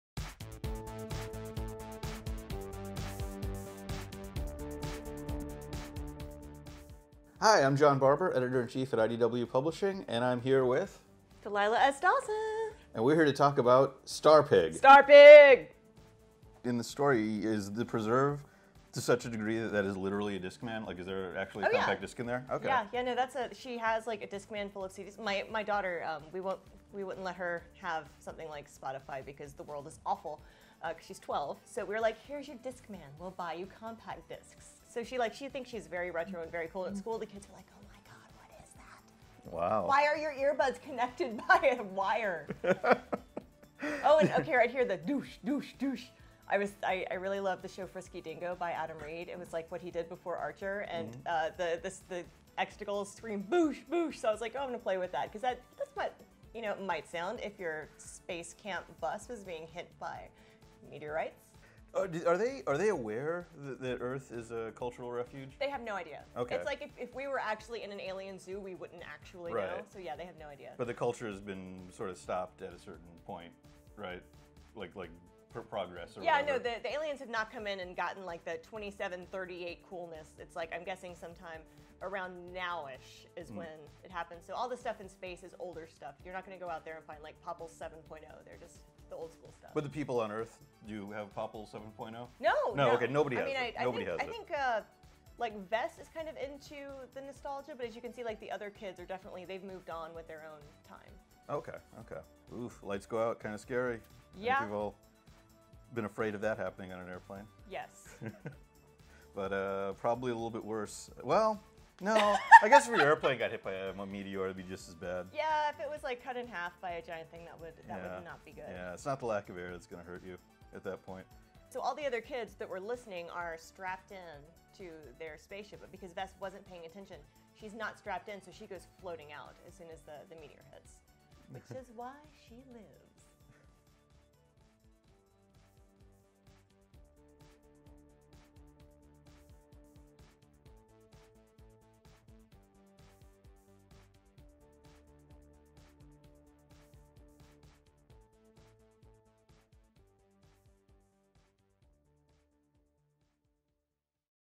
Chat About Art & Stuff with Delilah S. Dawson for Star Pig!
Two castaways. One goal. Zero in common.

New York Times Bestselling author Delilah S. Dawson sits down with IDW Editor-in-Chief John Barber to discuss the art and other fun facts about her new series, STAR PIG for TardigradeTuesday!

Witness the beginning of an extraordinary friendship and an incredible journey home with the first issue, on sale in July!